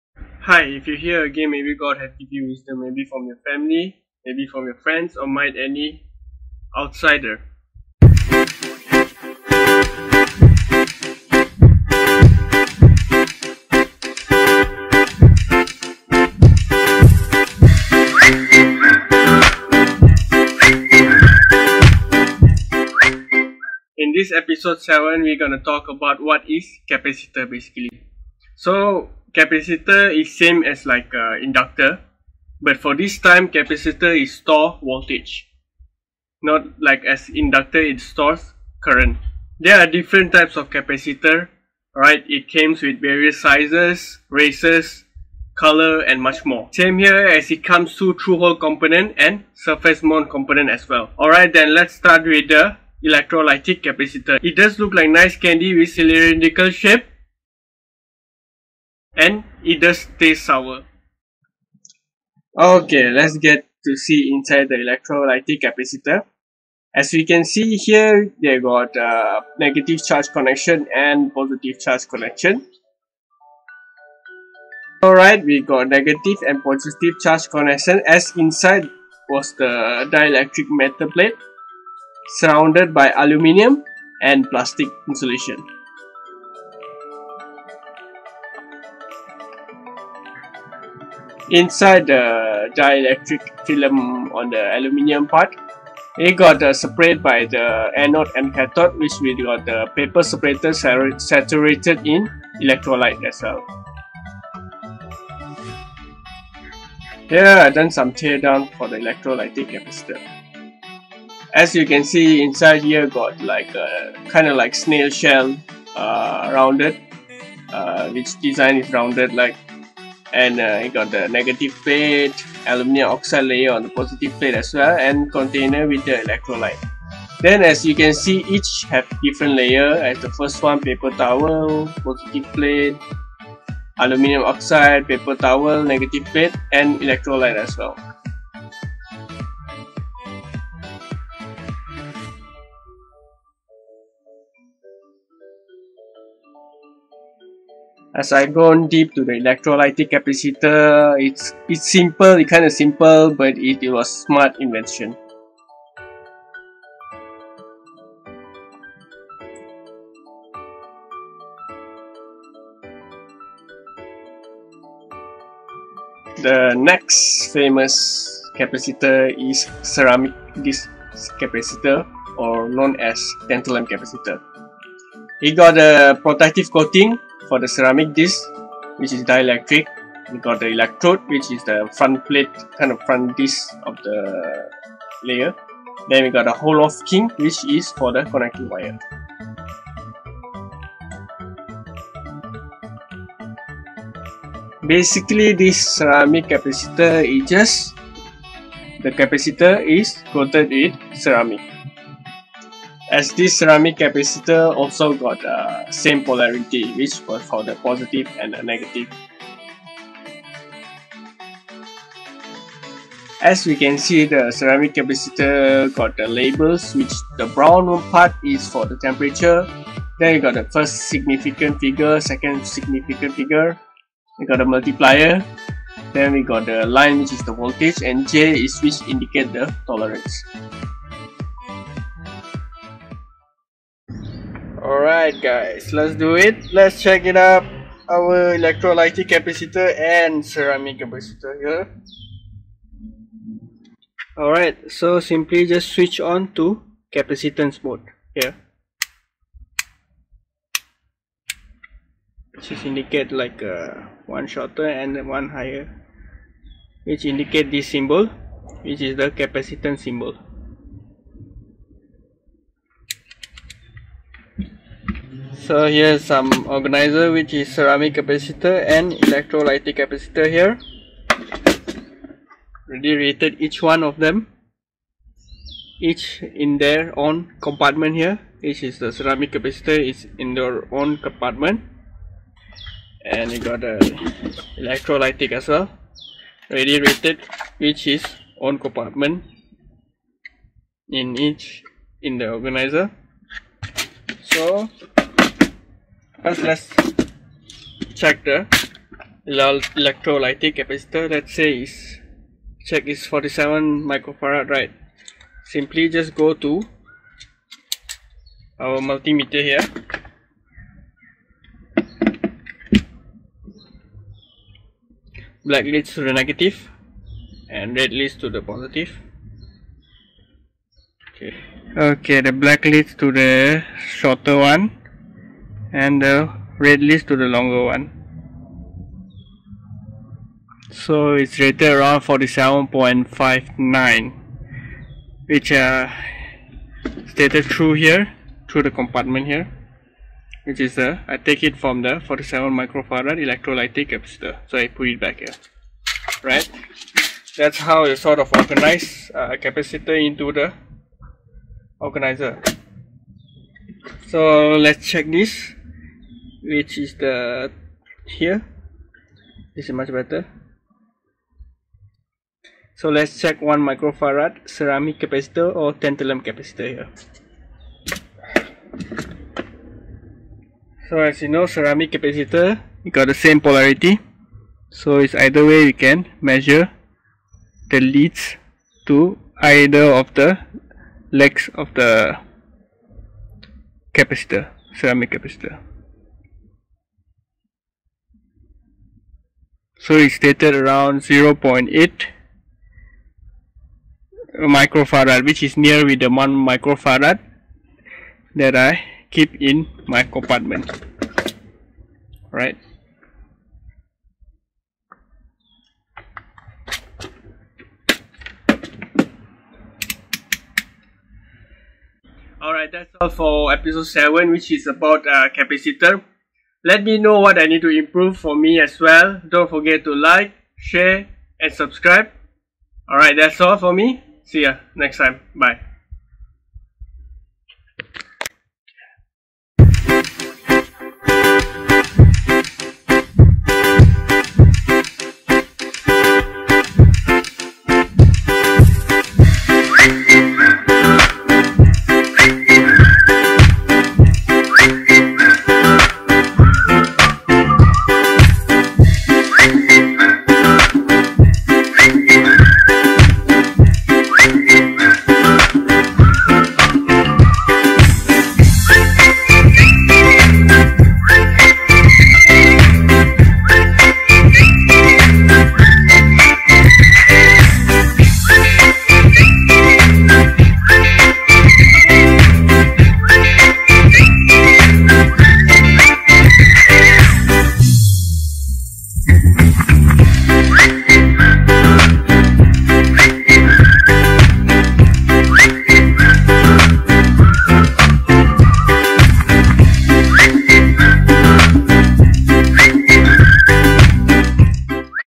Understanding Electronic Component 07: Capacitor
It's like inductor but it stores voltage in capacitance value.

0:00 Intro
0:24 Capacitor Briefing
1:01 Inside Capacitor
3:54 How to read capacitance value of Capacitor using Multimeter
6:52 End